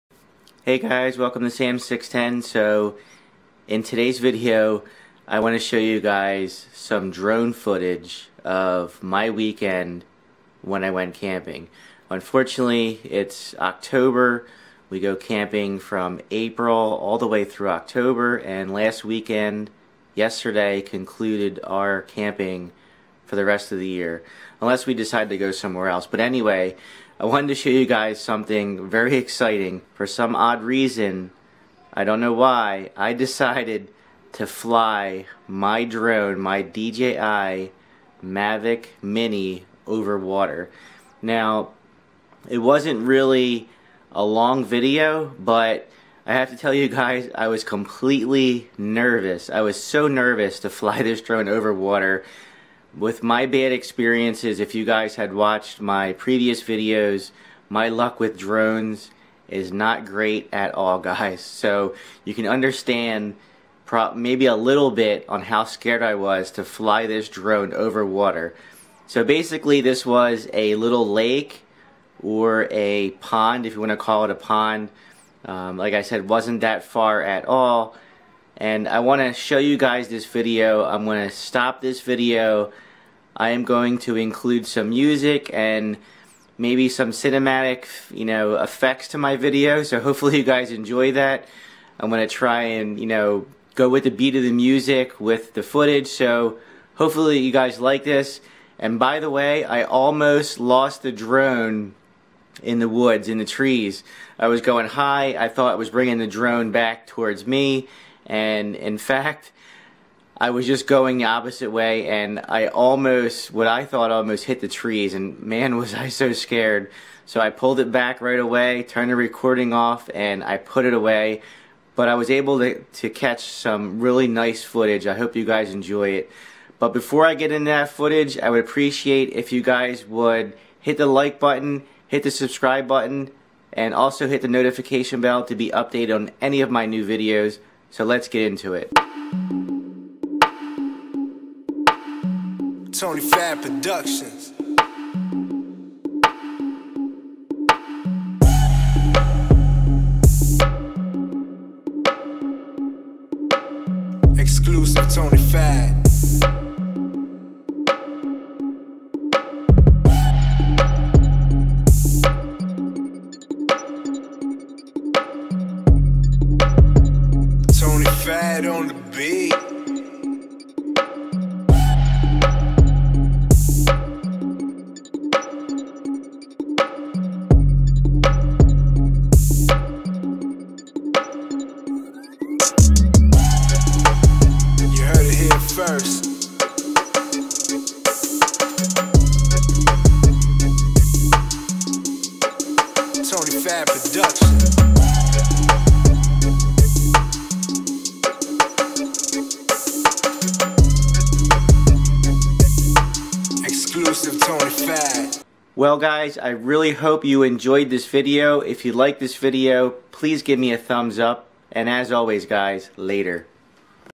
DJI Mavic Mini Over Water For The First Time
I finally decided to take my DJI Mavic Mini drone over water for the first time ever. I was really scared to take my drone over water. I am glad my drone did not crash flying over water.